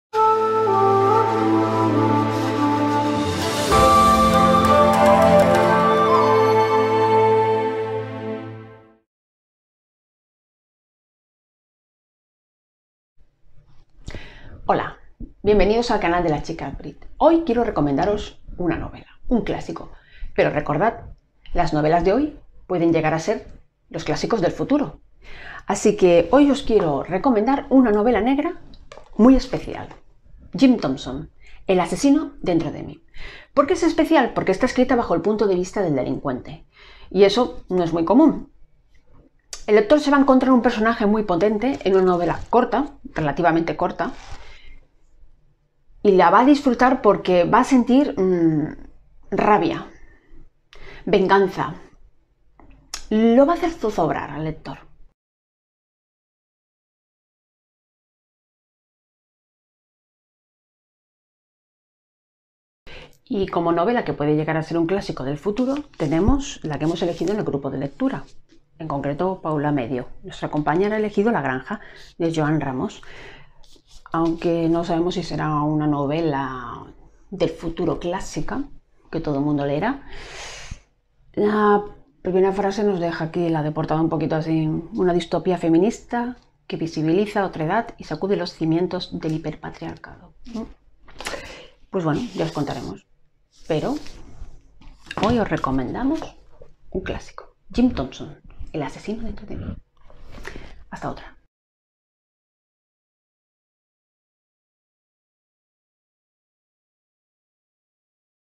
Sant Jordi 2020. Vídeo 2.
Vídeo 20. Segundo vídeo breve dedicado al día especial de Sant Jordi en Cataluña que nos perderemos debido al confinamiento que sufre España.
Recomendaciones de dos seguidores en Twitter.